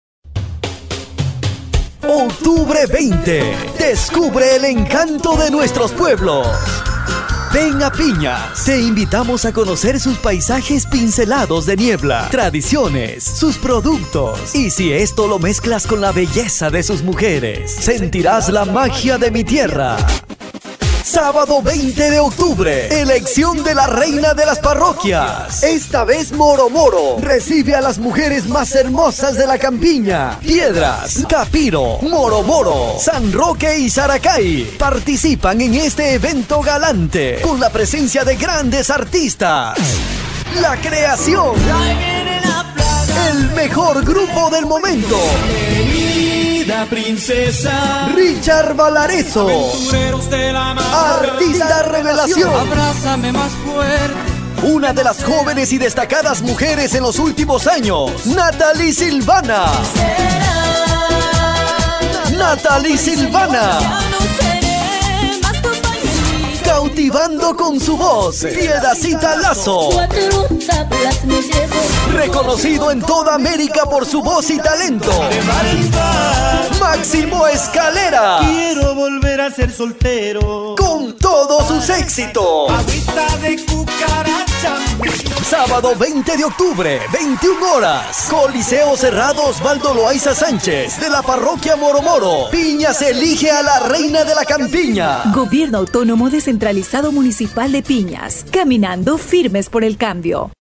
Spot de invitación a la elección de la Reina de las Parroquias
Este sábado 20 de Octubre en la parroquia Moromoro se elige a la Reina de las Parroquias de Piñas, evento que estará complementado con la presentación de grandes artístas